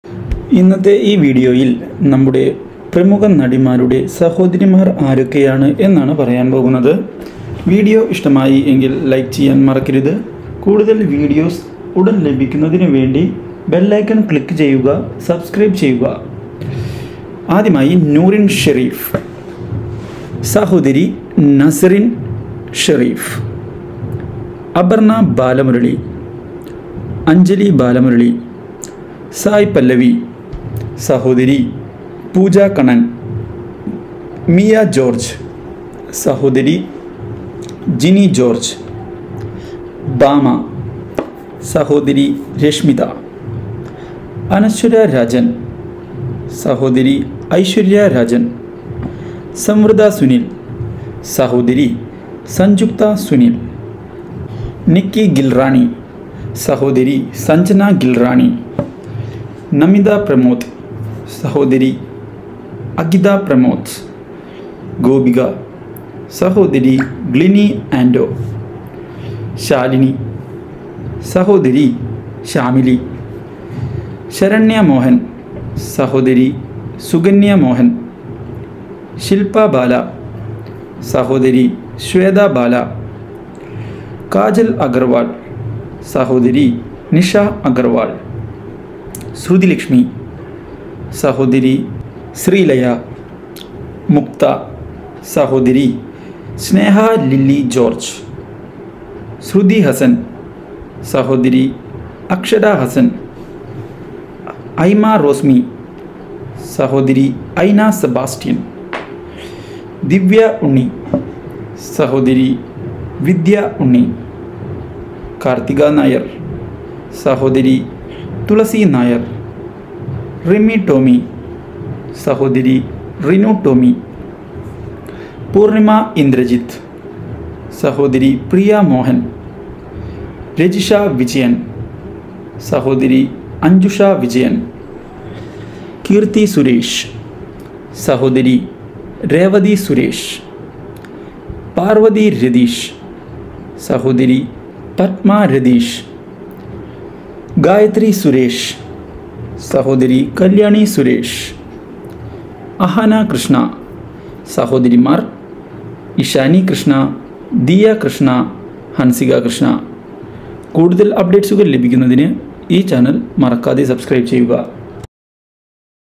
ഞെട്ടിപ്പോകും 😲 മലയാള നടിമാരുടെ യഥാർത്ഥ സഹോദരിമാർ- REAL LIFE SISTERS of Malayalam film actress
This video talks about the sisters of Malayalayam actress, Malayalayam actress sisters are included in the video. This video talks about sisters of all Malayalayam actress who is active in Malayalam film industry.The video includes the sisters of sai Pallavi,sai Pallavi sister is Pooja kannan, sister of actress noorin shareef, sister of actress namitha pramod, sister of actress Aparna balamurali, sister of actress mia George, actress Bhama sister is reshmitha, actresssamvritha Sunil sister is sanjukta sunil, actress Nikki gilrani sister is Sanjana gilrani,actress namitha pramod sister is akhitha pramod, actress Gopika sister is glini anto, etc. The video includes sister of famous Malayalayam actressess